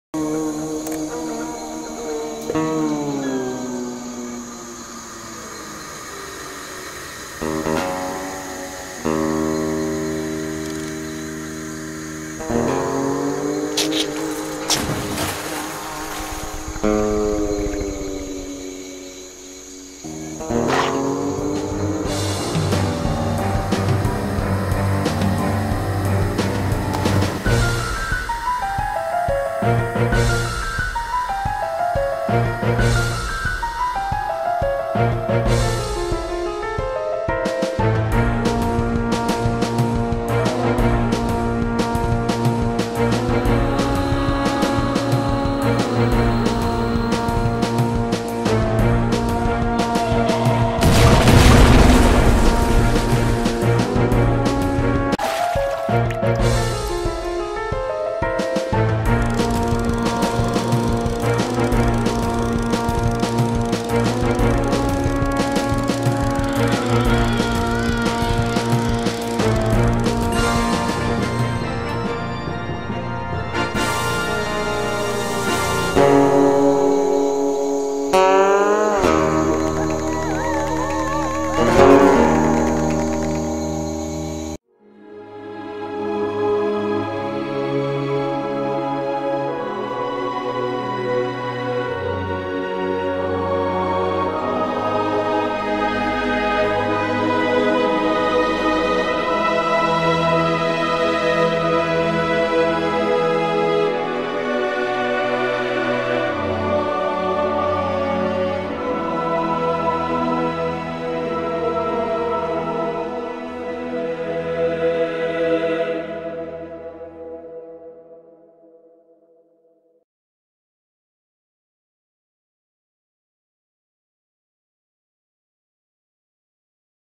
Winning The Nightmare Fishing Tournament
How it feels to chew 5Gum -- I mean, what it looks like to win the Nightmare Fishing Tournament.

Footage is of the last round ( The Pink Devil ) getting reeled in at 1400 lbs even, securing my top spot in the leader board against the computer competitors . As a result, my corpse won the tournament and was resurrected to the Land of the Living